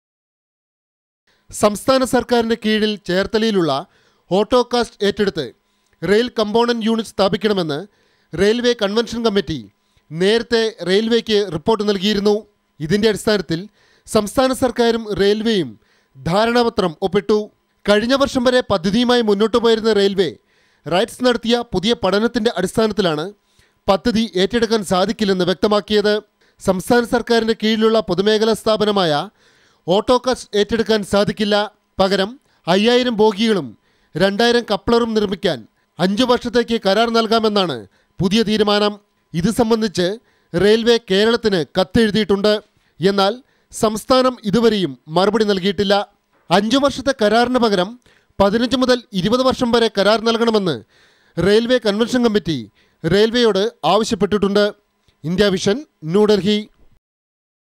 Railway not ready to take over Rail component unit project in Alappuzha
ആലപ്പുഴയില്‍ സ്ഥാപിക്കാനിരുന്ന റെയില്‍ കംപോണന്റ് യൂണിറ്റ് റെയില്‍വെ ഏറ്റെടുക്കില്ല. ഇതു സംബന്ധിച്ച് റെയില്‍വെ കേരളത്തിന് കത്തെഴുതി. റൈറ്റ്‌സ് നടത്തിയ പുതിയ പഠനത്തിന്റെ അടിസ്ഥാനത്തിലാണ് റെയില്‍വെയുടെ തീരുമാനം.